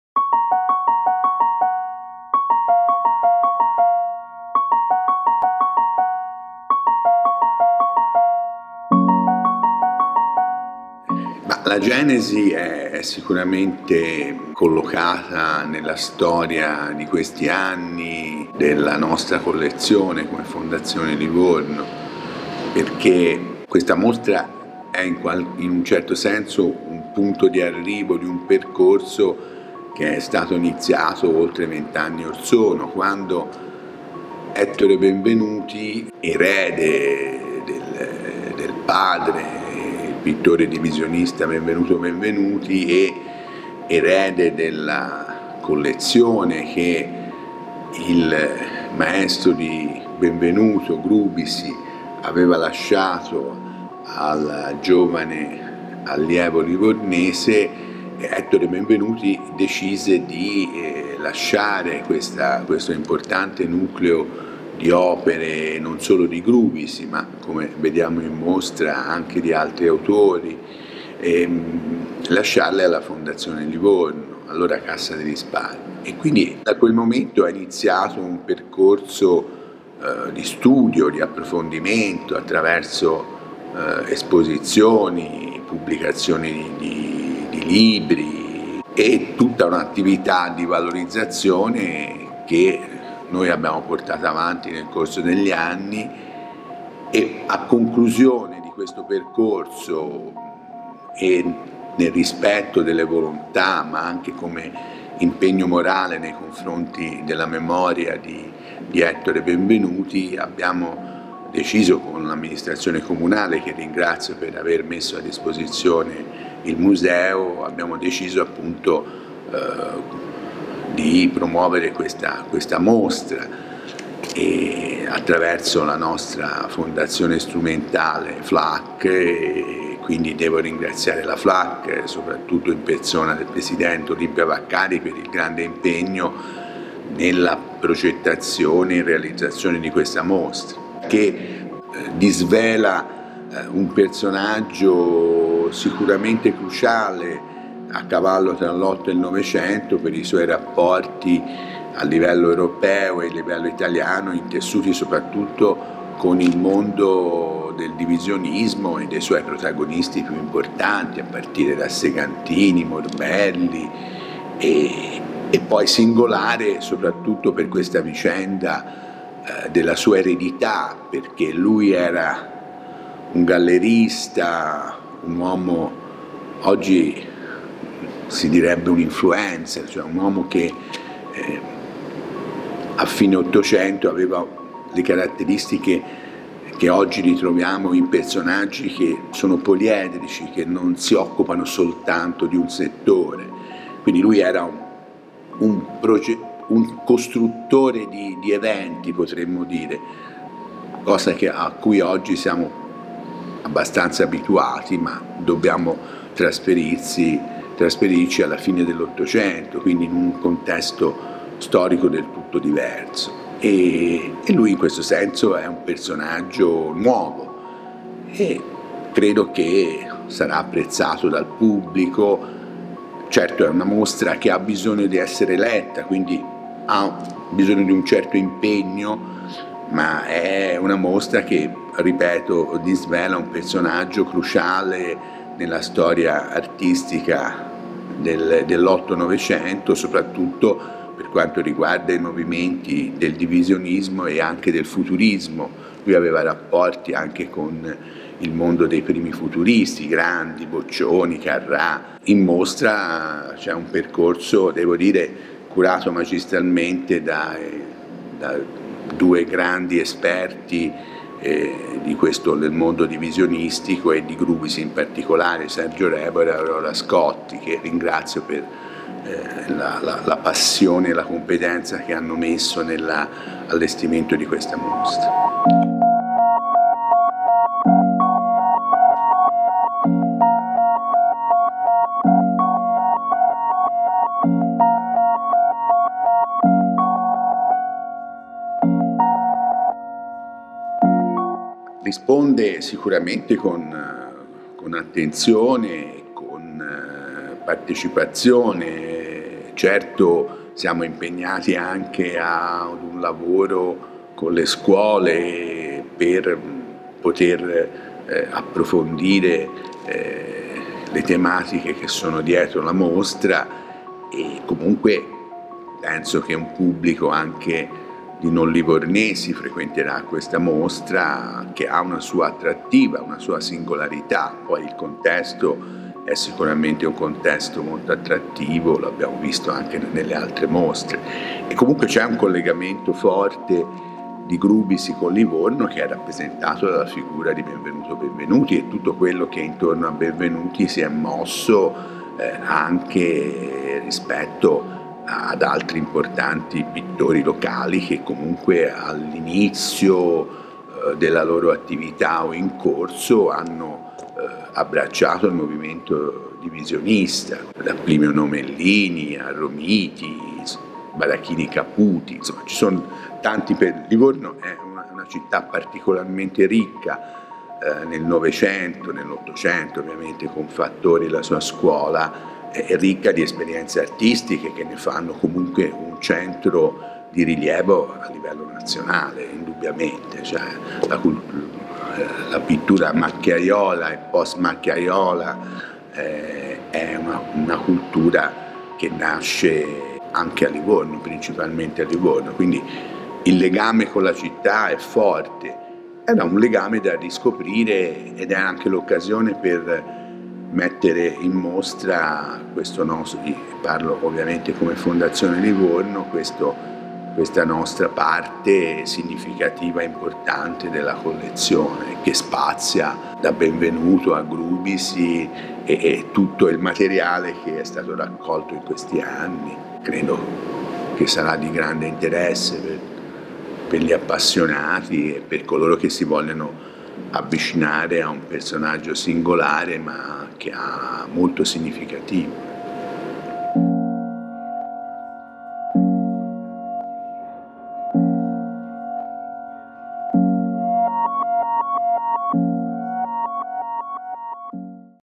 Vittore Grubicy De Dragon, intervista a Luciano Barsotti
Livù - La web tv di Livorno

L'avvocato Luciano Barsotti, presidente di Fondazione Livorno, racconta la genesi e l'elaborazione della mostra "Vittore Grubicy De Dragon. Un intellettuale artista e la sua eredità: aperture internazionali tra divisionismo e simbolismo". Una figura affascinante, celebrata in questa mostra curata da Sergio Rebora e Aurora Scotti, che trova spazio al Museo della Città di Livorno dall'8 aprile al 10 luglio 2022.